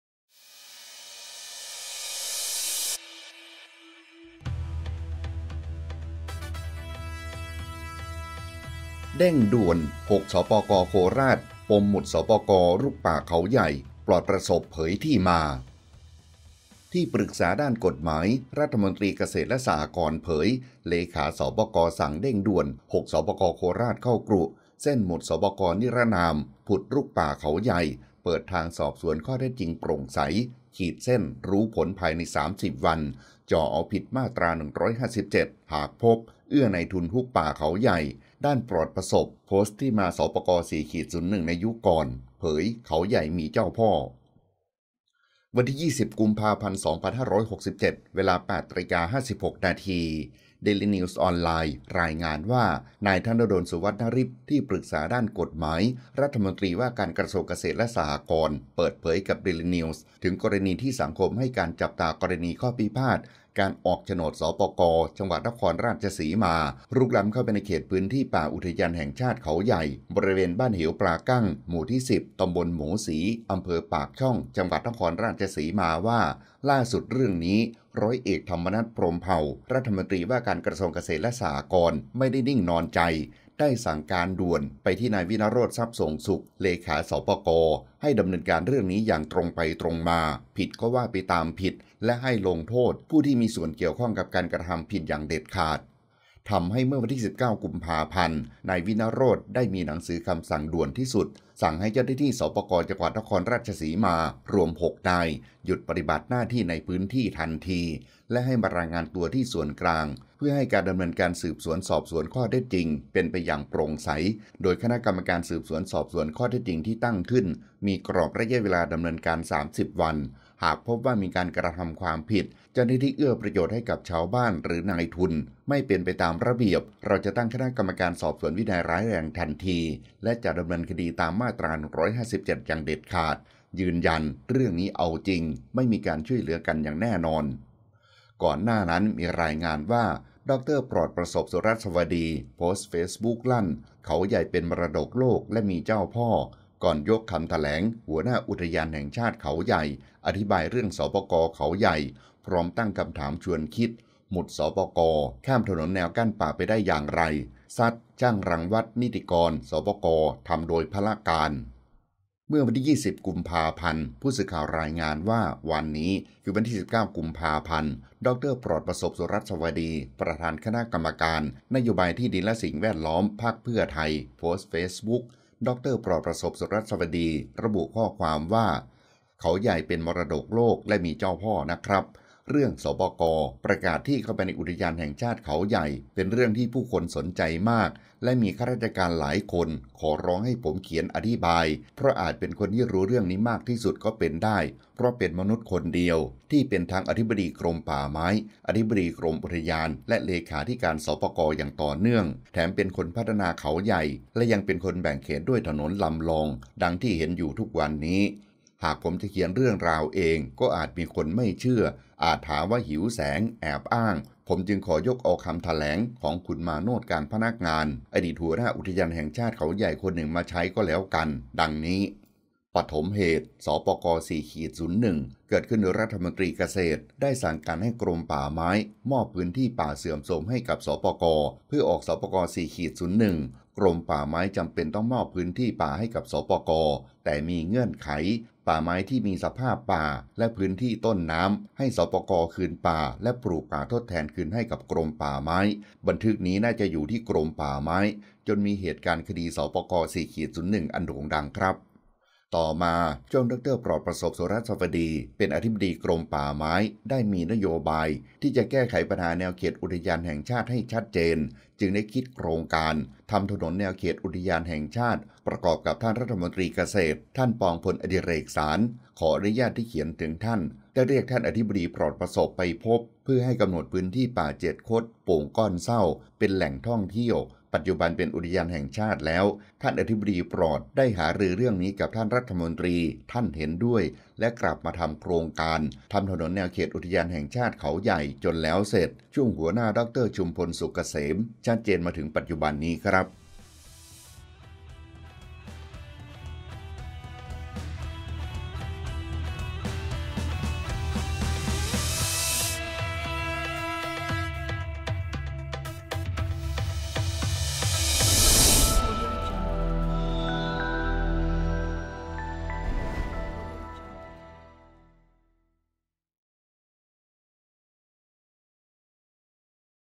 เด้งด่วน! '6 สปก.โคราช' ปมหมุด สปก.รุกป่าเขาใหญ่ 'ปลอดประสพ' เผย ที่มา
ขอบคุณที่มา : dailynews